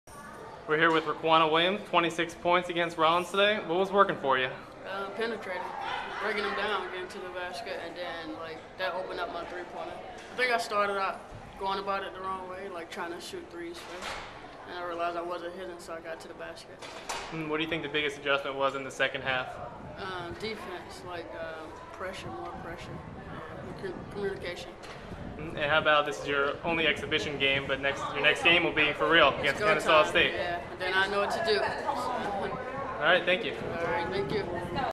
Miami Guard Riquna Williams
Williams speaks after exhibition win at Rollins.